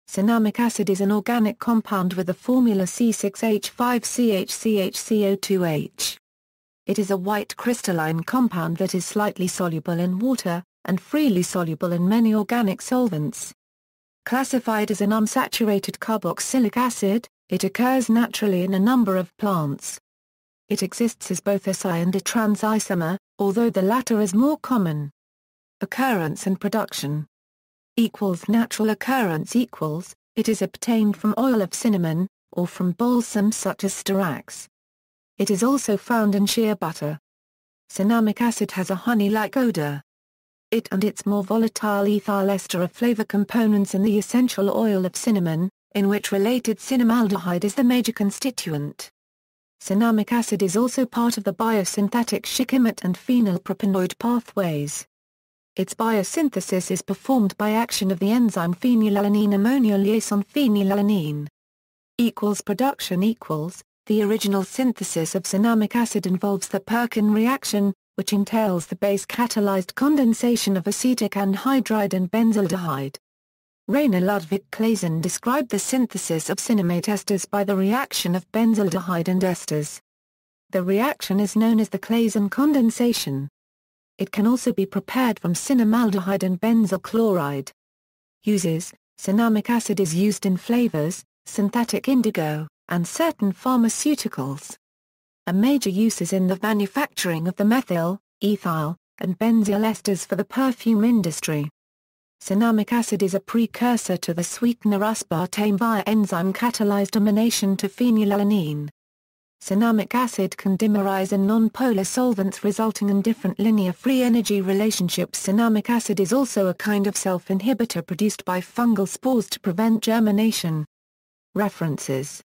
Cinnamic acid
Cinnamic acid is an organic compound with the formula C6H5CHCHCO2H. It is a white crystalline compound that is slightly soluble in water, and freely soluble in many organic solvents. Classified as an unsaturated carboxylic acid, it occurs naturally in a number of plants. It exists as both a cis and a trans isomer, although the latter is more common.